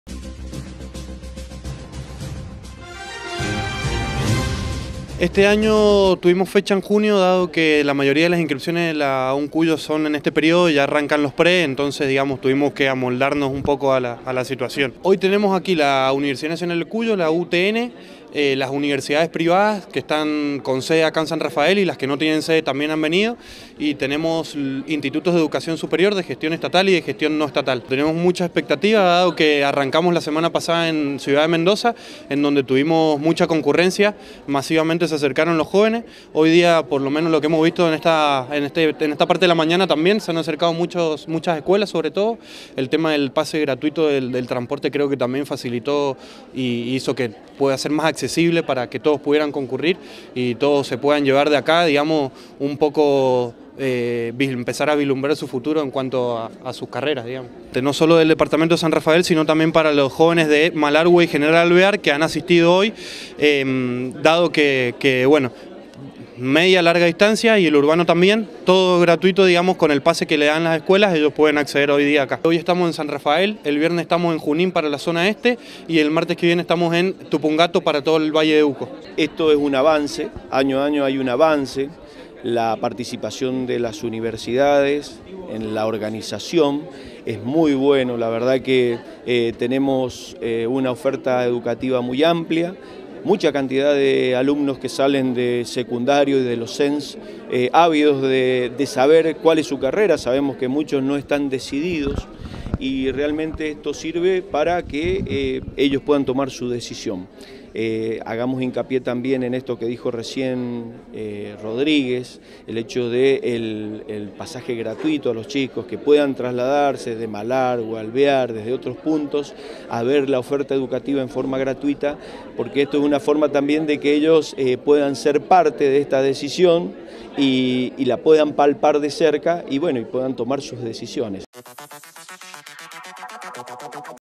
PRÁMBULO AL BOLETO ESTUDIANTIL
La Universidad de Cuyo, en junto a otras casas educativas públicas y privadas, mostraron que carreras pueden estudiarse en San Rafael. Asimismo, la DGE otorgó pases de transporte gratuitos para la asistencia de los estudiantes que viven en zonas más alejadas.